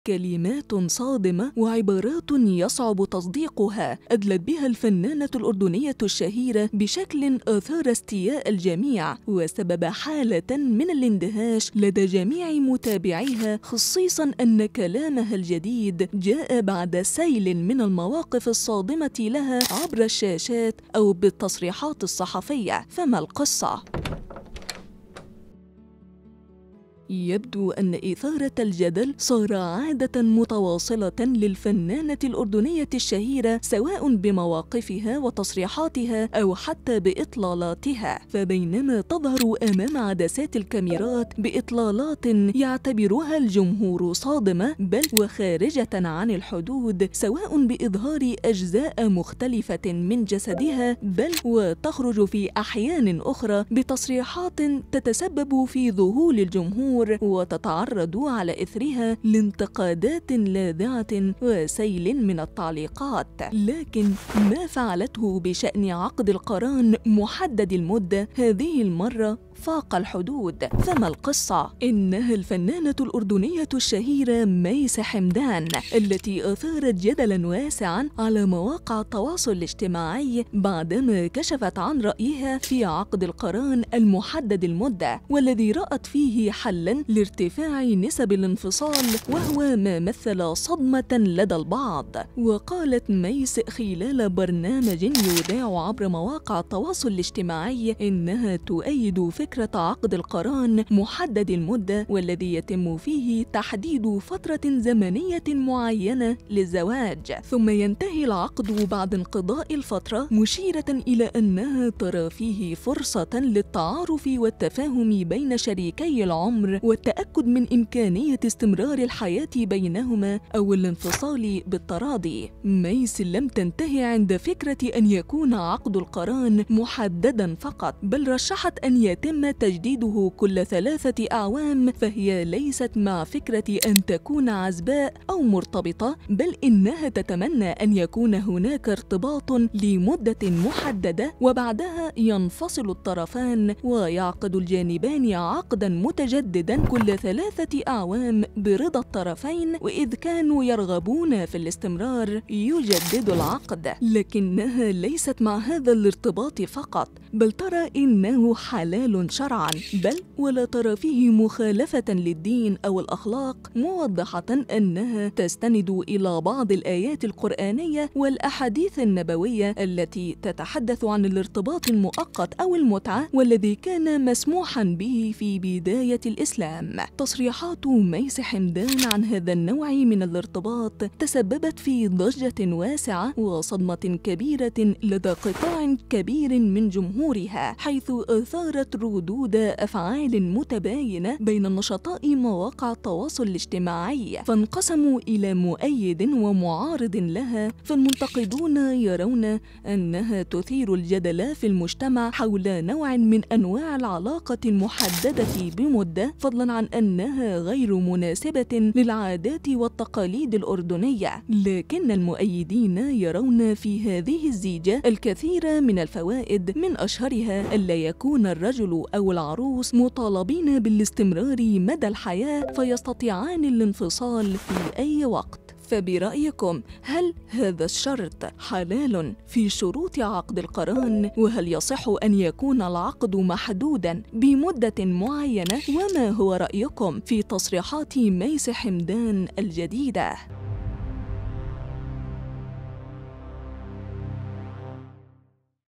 فنانة عربية شهيرة تثير الجدل بشأن طرحها لعقد قران جديد.. لن تصدق ماذا قالت عن العقد الغامض!
يبدو أن إثارة الجدل صار عادة متواصلة للفنانة الأردنية الشهيرة، سواء بمواقفها وتصريحاتها أو حتى بإطلالاتها.